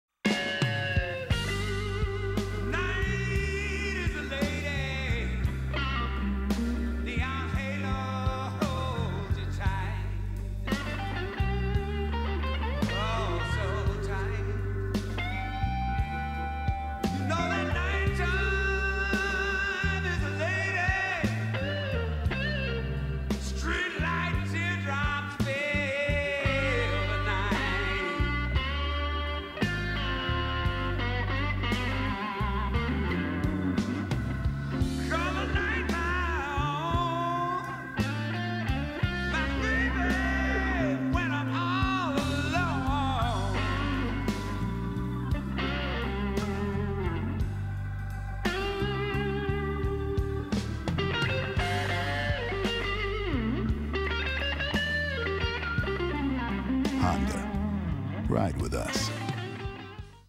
Honda Blues Spot
This Honda Motorcycle TV ad was done by Matthews-Griffith Music. The players were Mitch Holder/guitars, Peter Erskine/drums, Nathan East/bass, Randy Waldman/B3.